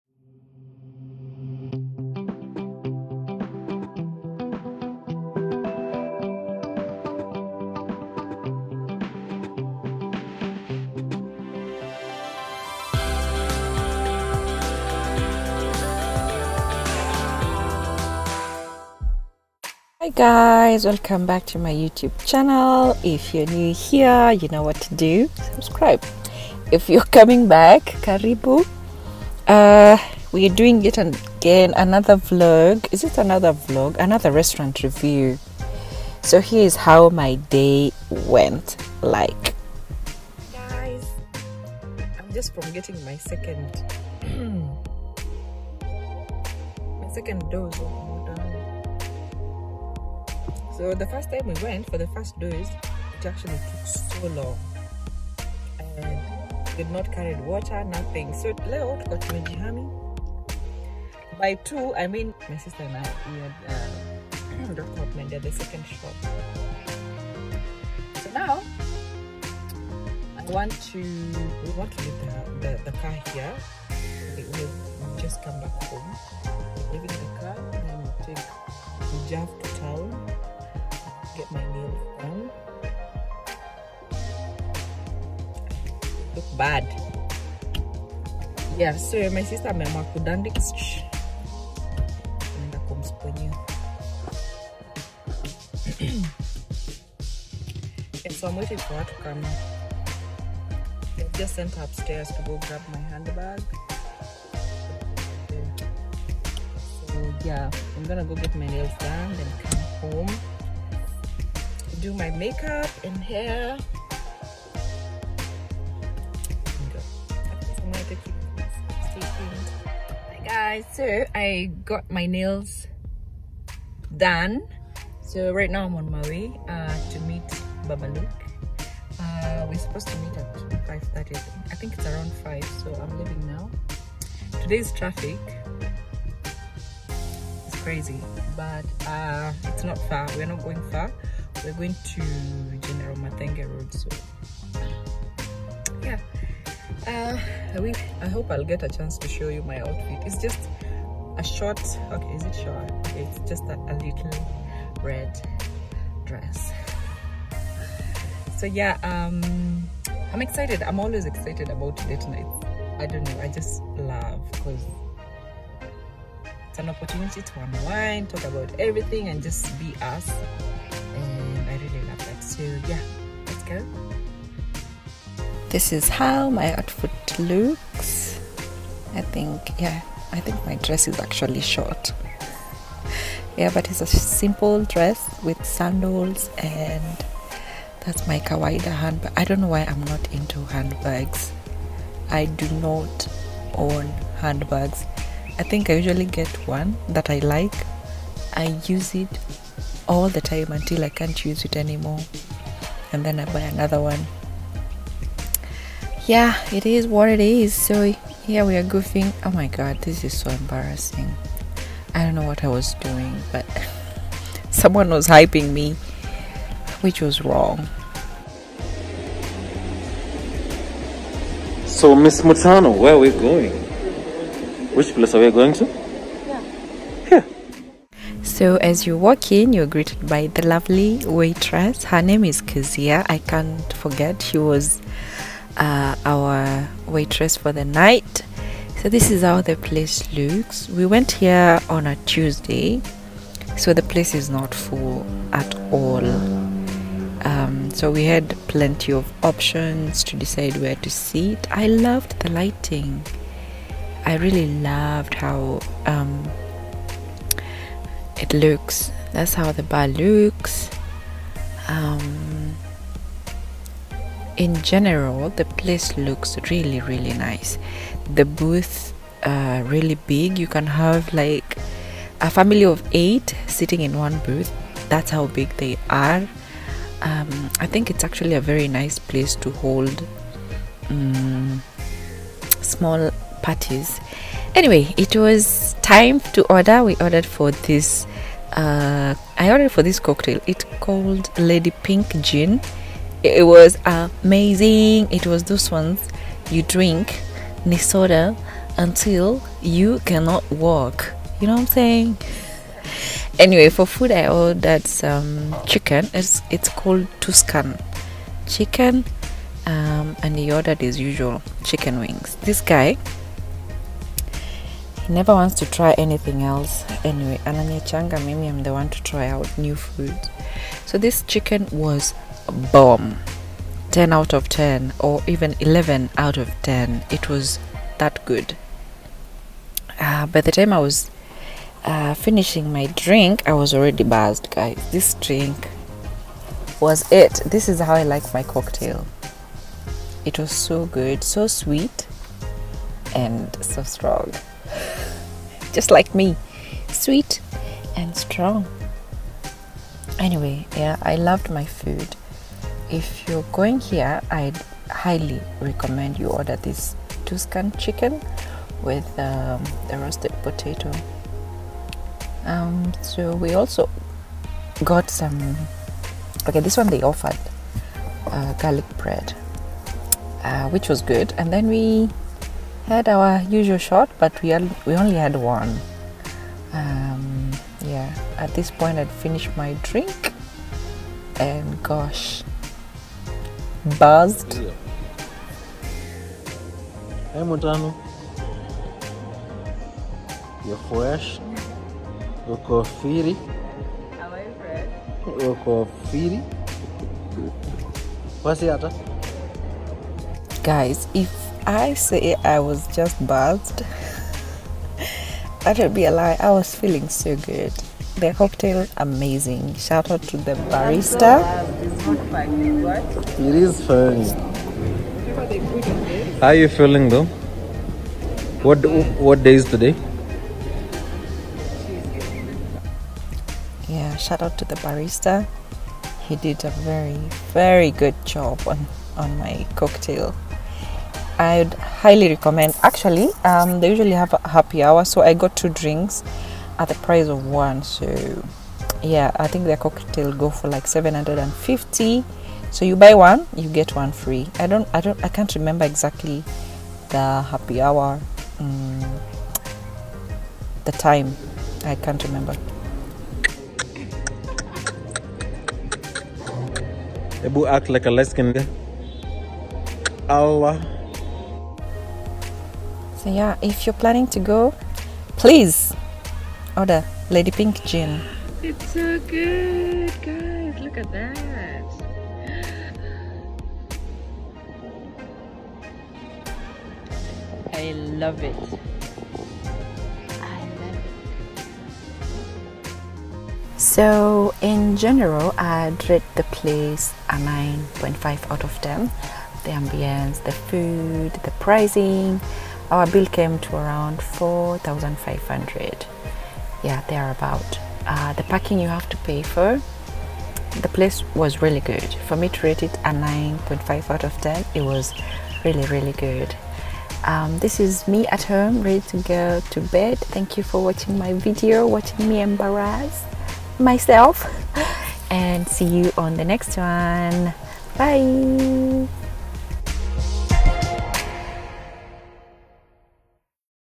DATE NIGHT AT THE COPPER IVY|| embarrazzed myself || Ms. MUTANU
We went to The Copper Ivy and we absolutely loved it.
The food was amazing, the drinks bomb ass, the ambiance 🙌🏽🙌🏽.
Please enjoy the vlog.